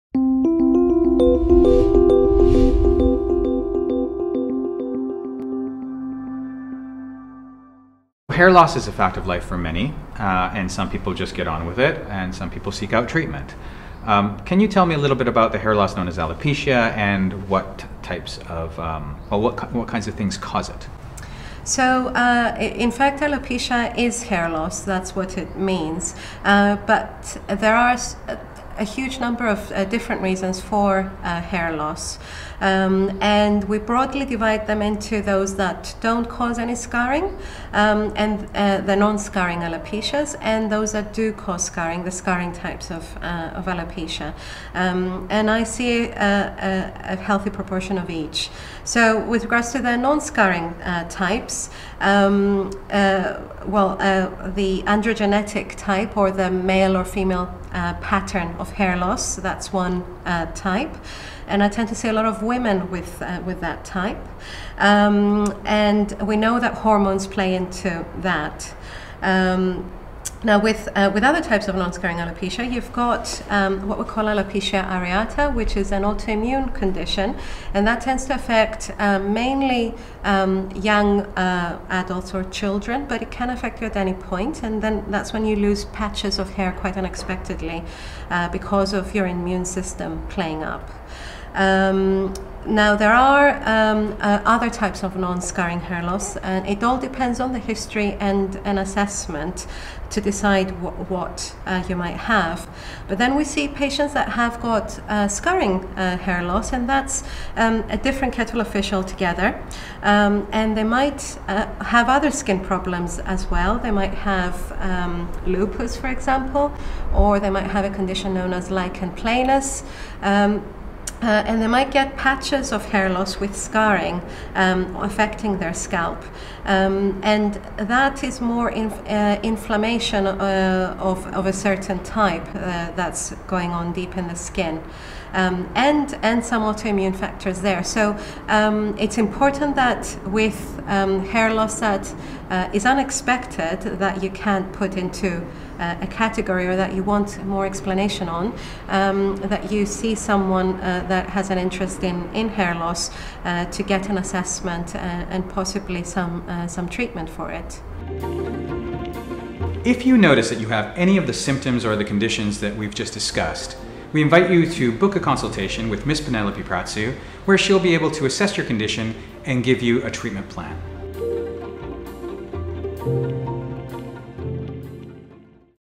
What is hair loss (alopecia) and what causes it?
To learn more about hair loss (alopecia) and how we treat it, please visit our Alopecia is hair loss. We divide alopecia into two types: scarring and non-scarring. If you experience unexpected hair loss, it's important to see a hair loss or dermatology specialist. I have a wide variety of treatment options at my disposal that can help treat your hair loss. These range from ointments to injections and tablets. Take the first step to feel in charge of your skin and book a free consultation